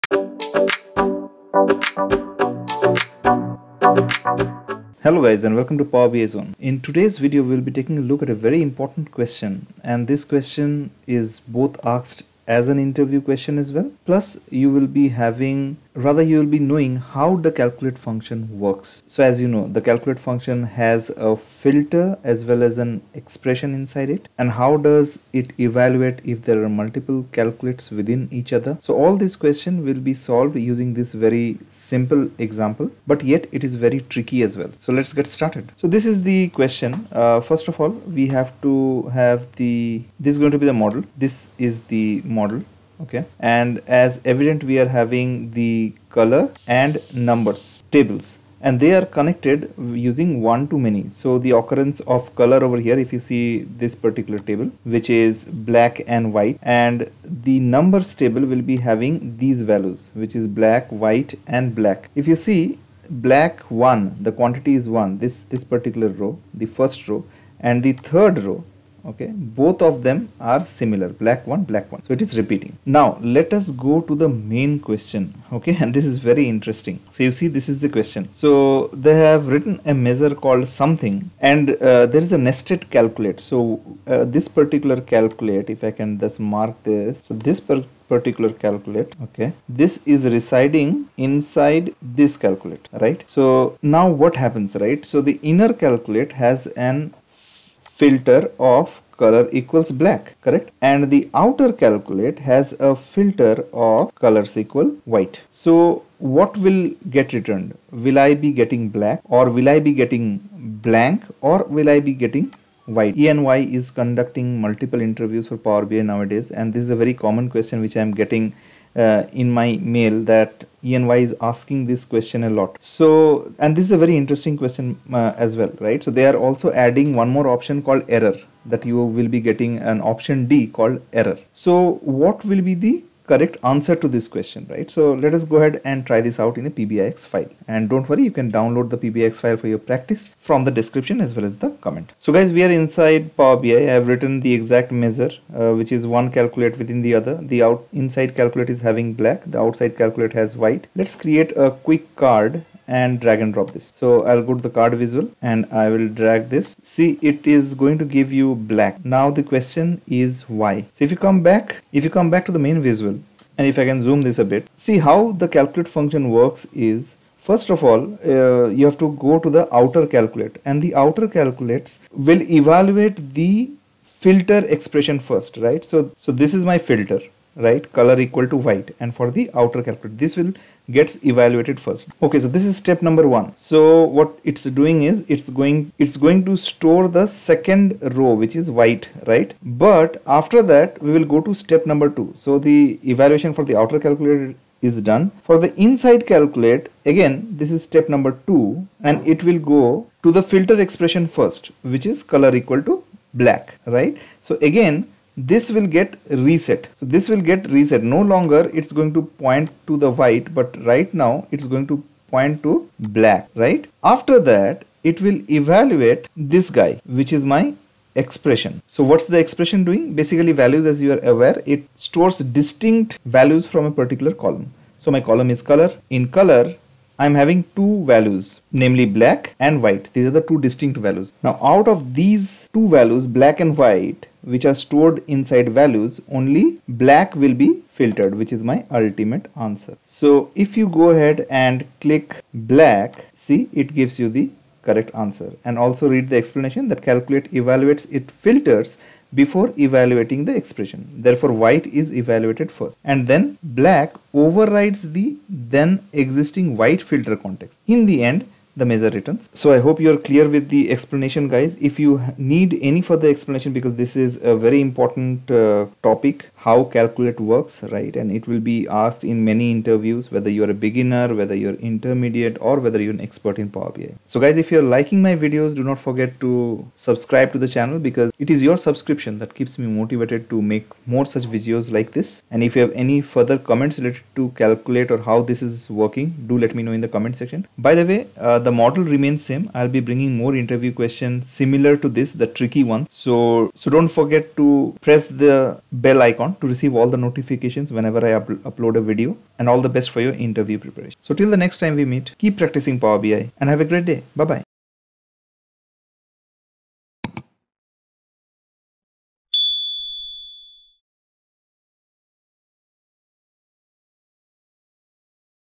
Power bi tricky interview questions | CALCULATE Dax made Easy
Hello Friends ,in todays episode we see a very interesting and tricky Question asked in E&Y and it involves the usage of the DAX Function called "CALCULATE".We have tried our level best in explaining the scenario and then explaining the CALCULATE function in the easiest way possible.
Hope you like the step by step explanation .
Consider the below measure written in DAX and has the usage of the CALCULATE function :
Measure =
CALCULATE(
    CALCULATE(VALUES(Color[Color]),Color[Color]="Black"),
Color[Color]="White")
the interviewer may ask what will be the output and the answer is Black .Since How the calculate function works is :
Step 1: it uses the outside CALCULATE first and then filters the result for FILTER ="White".This gets overridden in step 2:
Step 2:the inner CALCULATE gets evaluated and the previous value of White gets overridden bhy Black.

🔥🔥Learn Power BI through our vlog and Free Videos:🔥🔥